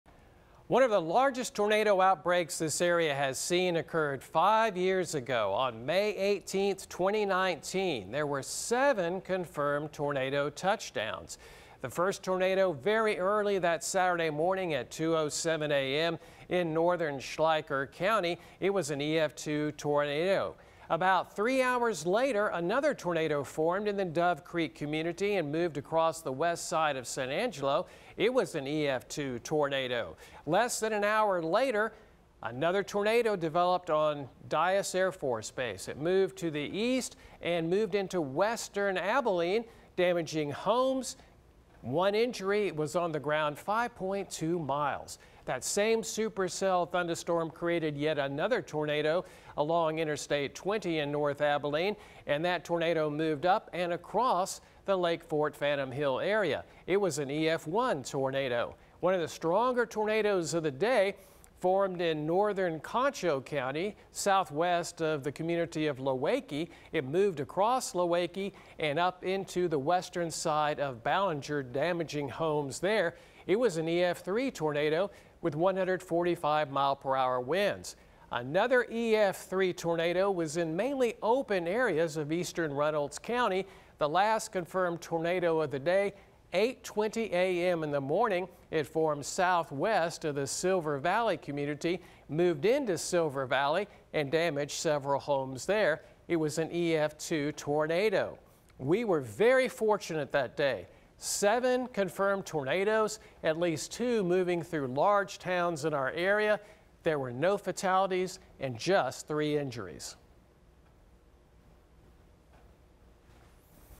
2019 tornado outbreak near Abilene, Texas
KXTS Meteorologist Mark Rowlett takes a closer look at the tornado outbreak that hit the region in 2019.